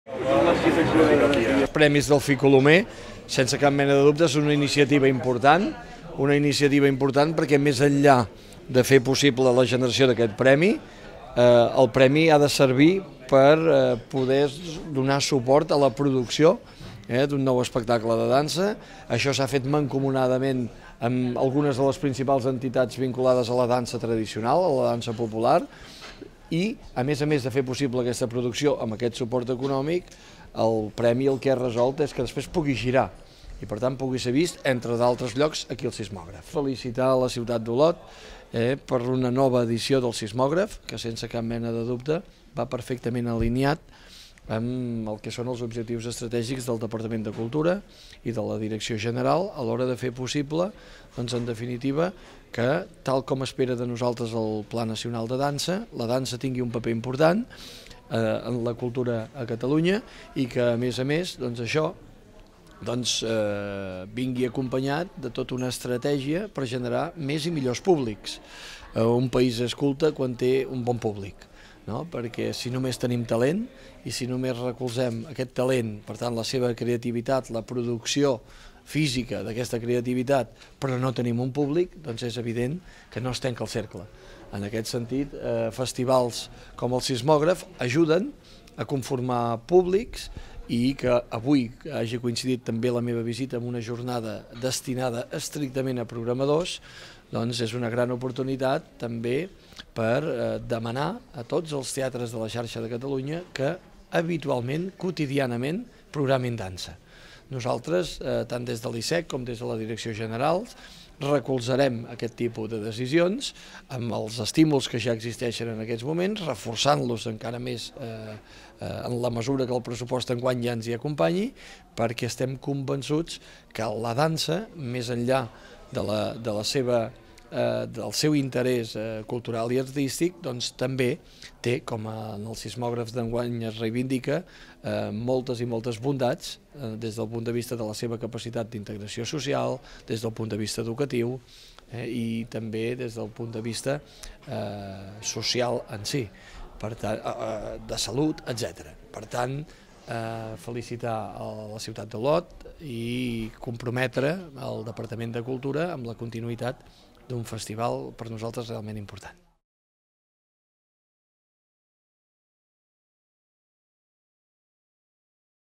Santi Vila presenta el Premi Delfí Colomé de suport a la dansa catalana tradicional
Santi Vila en la presentació dels Sismògraf d'Olot. Garrotxa